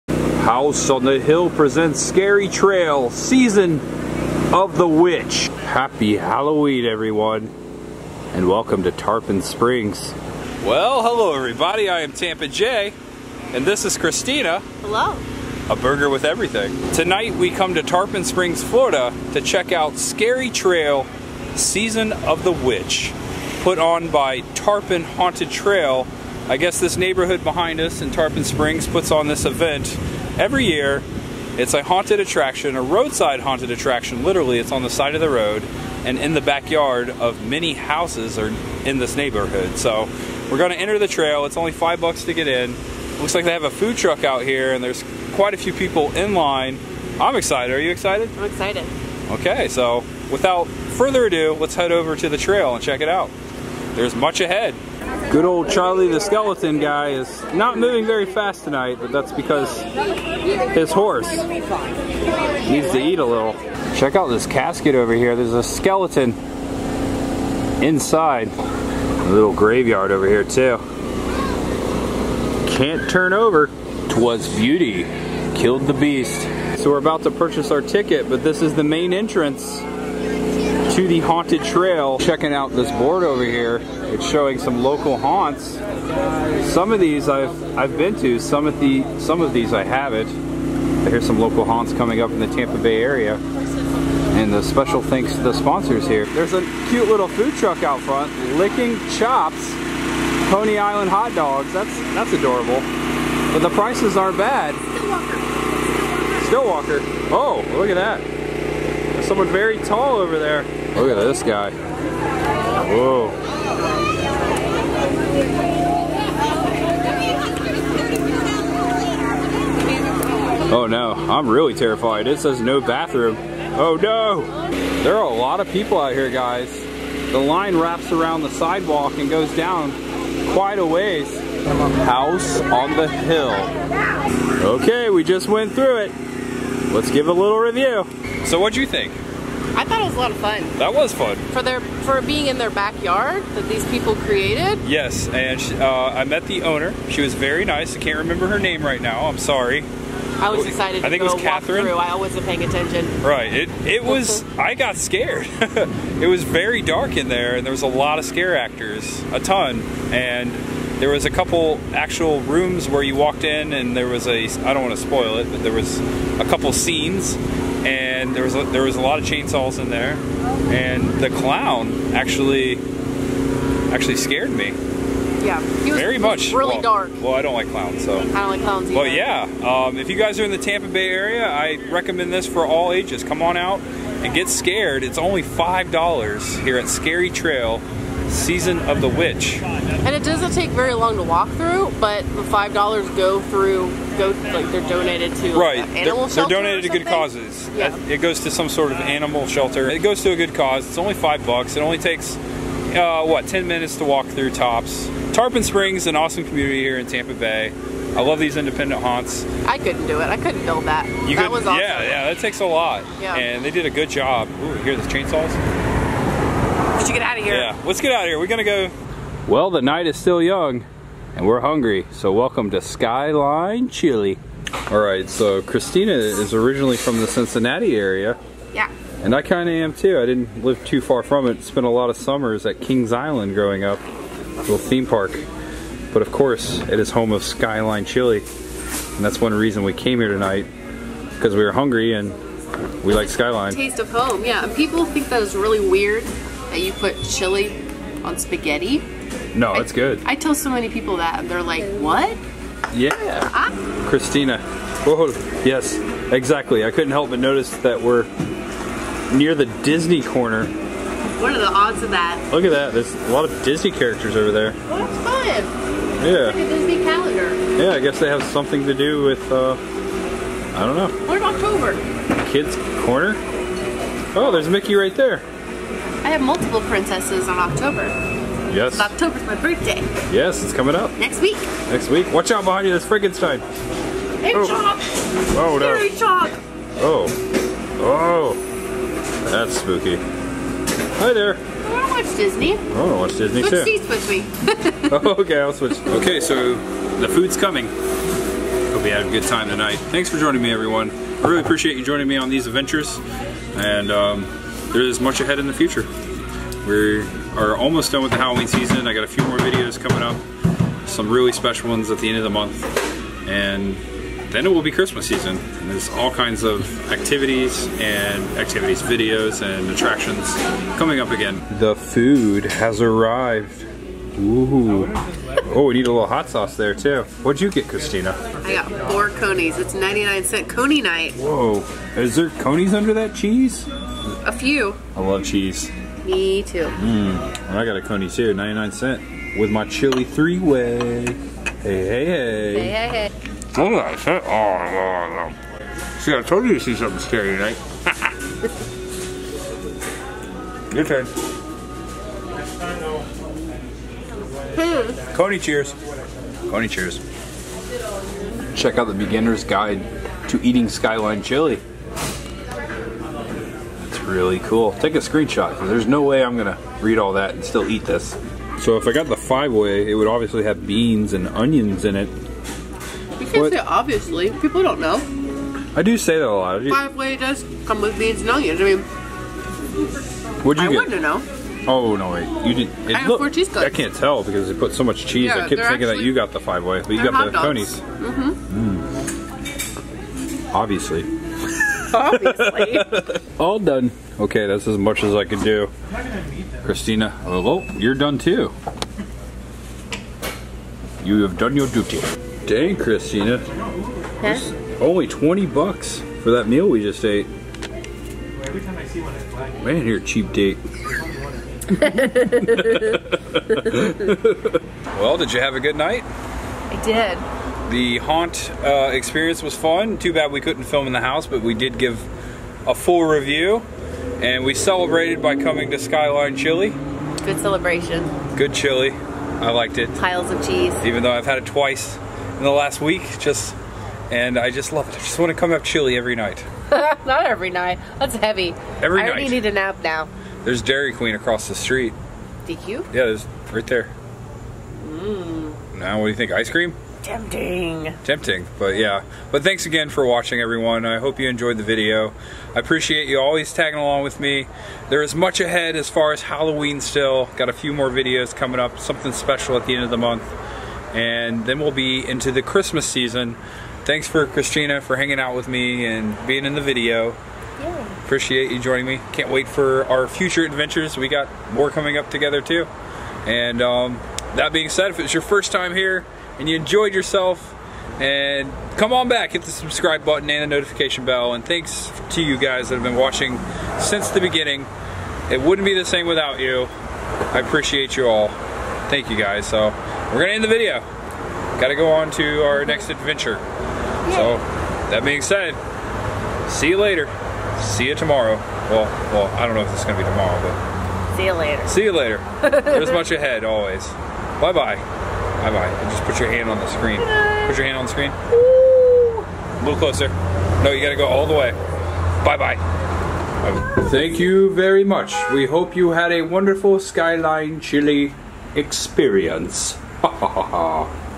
Florida HALLOWEEN fun at Scary Trail SEASON OF THE WITCH haunt Review & Dinner at SKYLINE CHILI
Halloween 2019 continues! Here is my review of Scary Trail: Season Of The Witch by Tarpon Haunted Trail is located on House On The Hill In Tarpon Springs Florida. Christina from A Berger With Everything joins me for the haunted house and dinner at Skyline Chili in Clearwater. There is much ahead!!

T shirts available at Spreadshirt